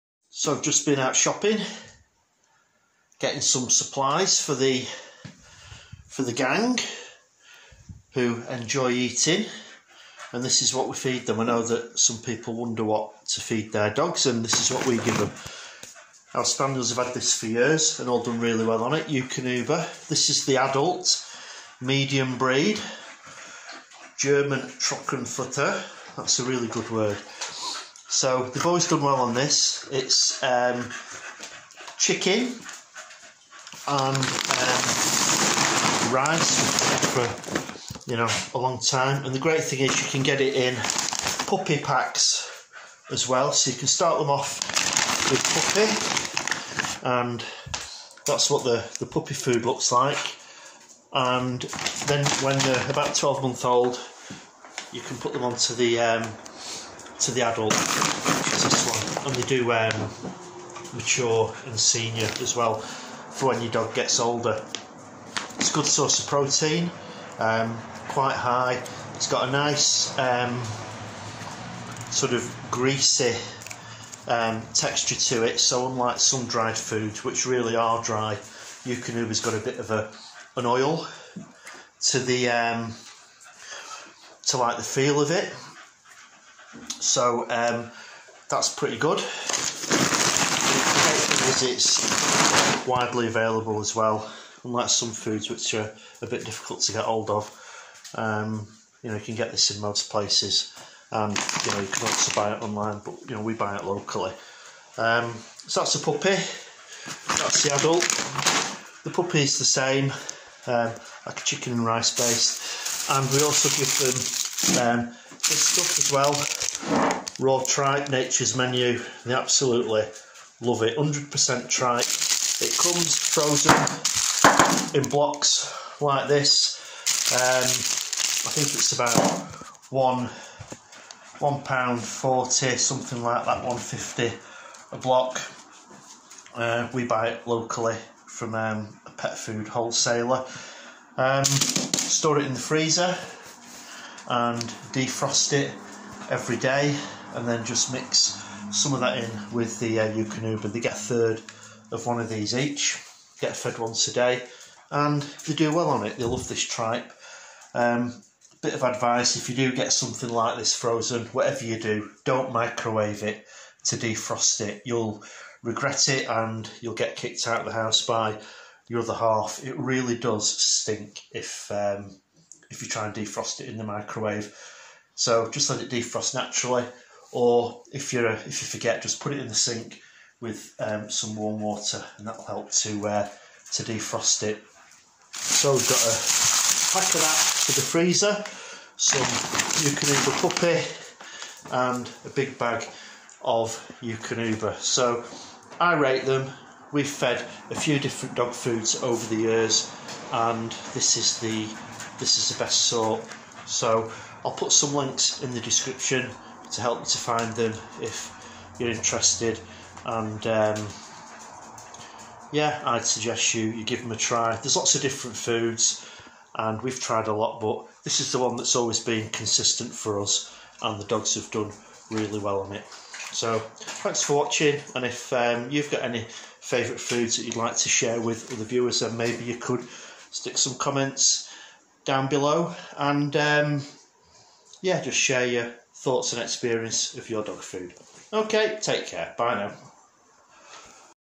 What to feed spaniels. What I feed my dogs.
There are a lot of choices available when you feed your dog so it's important to take your time and choose a food that will suit your pets and will stand the test of time as well as being readily available.

I feed Eukanuba which I've fed for 30 years to spaniels and mix this in with tripe that I buy in frozen blocks, and the dogs love it and do well on the combination.

Raw foods